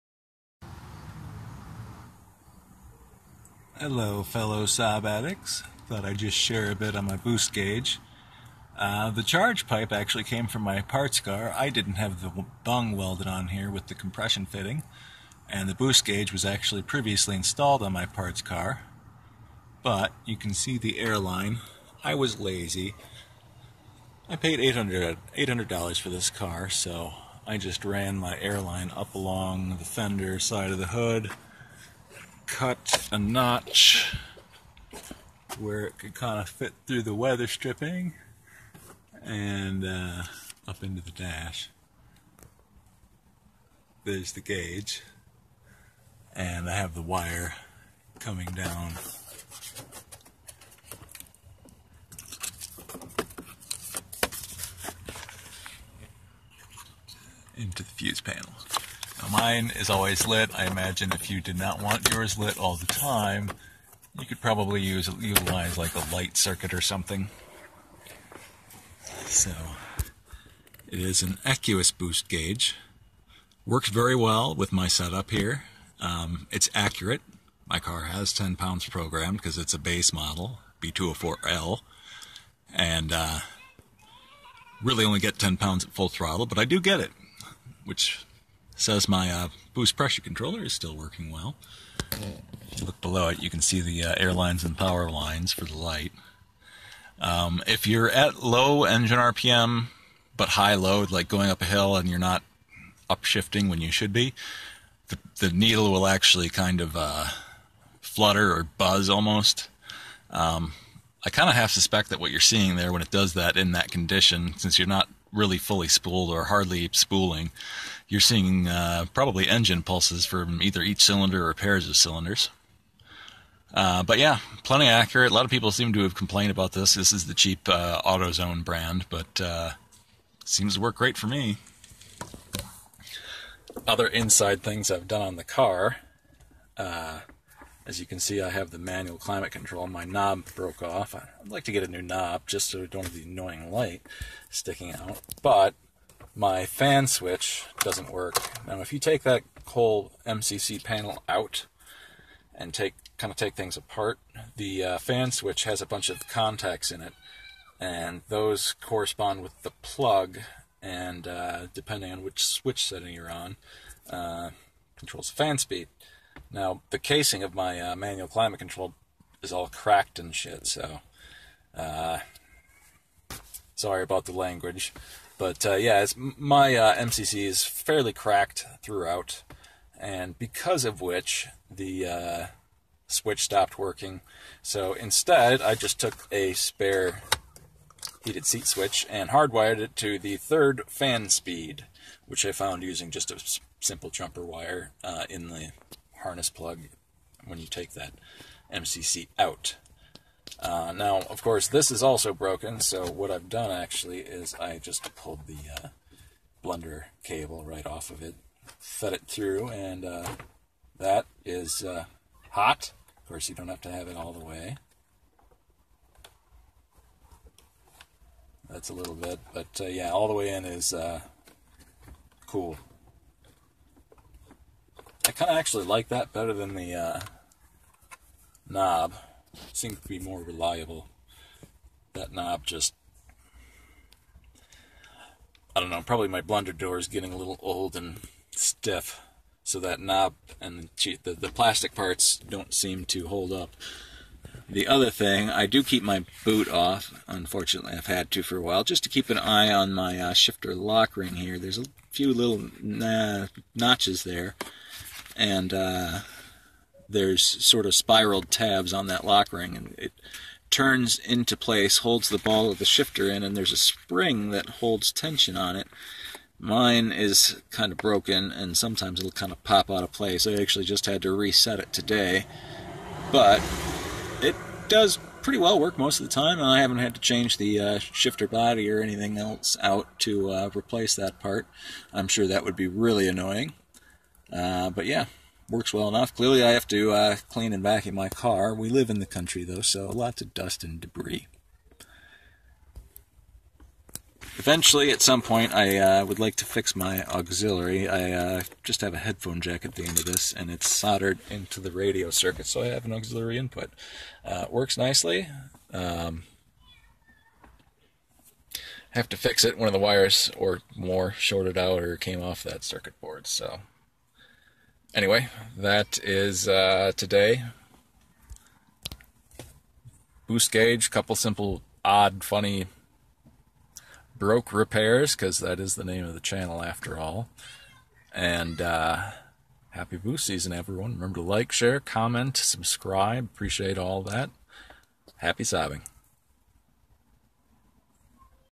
Equus Boost gauge and other bits
A short bit on how I've got my boost gauge installed.  Also some of a few ways I've retained function without really spending money.  Broke repairs in other words.  You'll have to excuse my somewhat dirty car.  I do live in the country with dust, dirt, and debris, and also only paid $800 for the car.